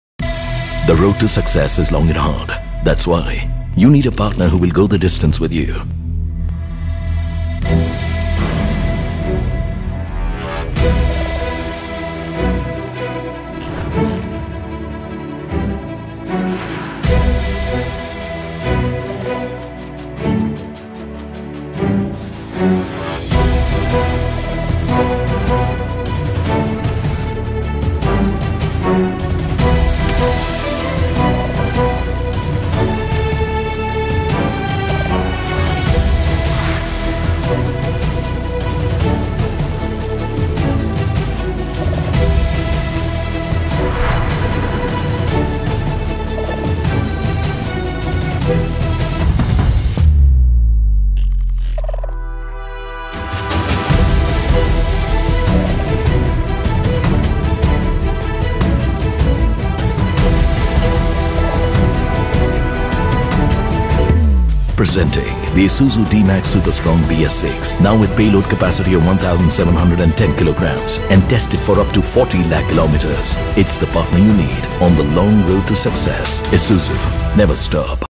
PartnerForSuccess ALL NEW ISUZU D-MAX
The wait ends here!

The road to success might be long and difficult but with the right partner by your side, success is inevitable!
Launching the all-new ISUZU D-MAX- Your only PartnerForSuccess in the long journey ahead!